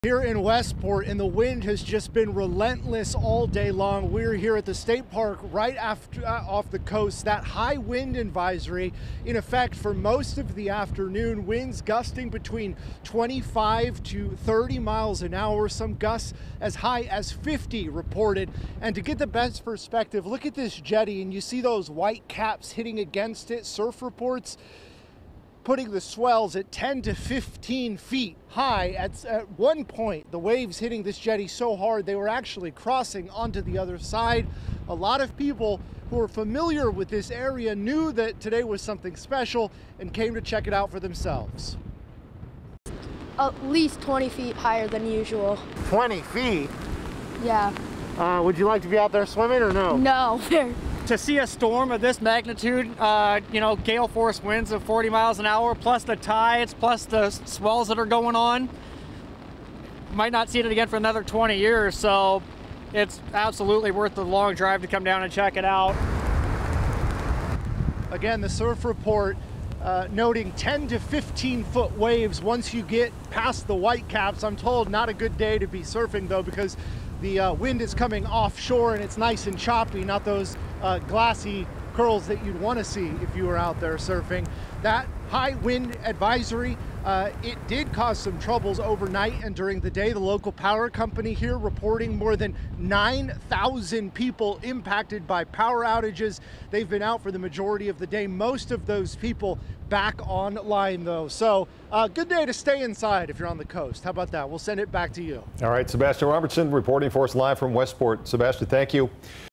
Heavy rain and high winds hit Washington coast
10 to 15 foot waves were reported in some areas, and wind in Queets got up to 81 mph.